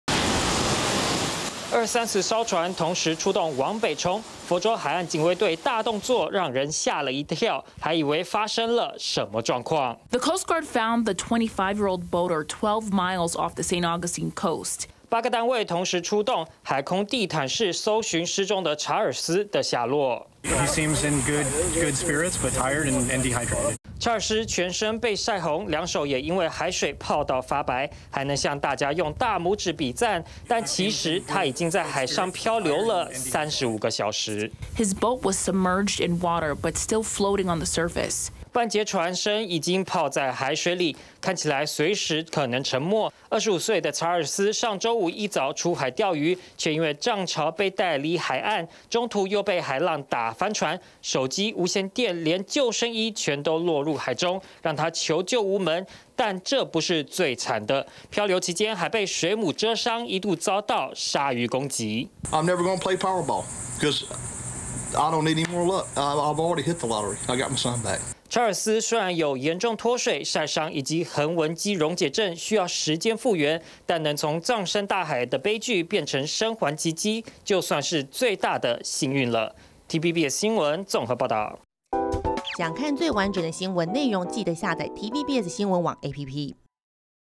漂流35小時!美佛州男出海翻船 奇蹟獲救｜TVBS新聞
美國佛州一名25歲男子出海釣魚卻失聯，原來是被漲潮海水帶離海岸，加上大浪打翻小船，手機、無線電全都落海，讓他在海上漂流整整35個小時才獲救，被發現時半截船身已經在水裡、相當驚險。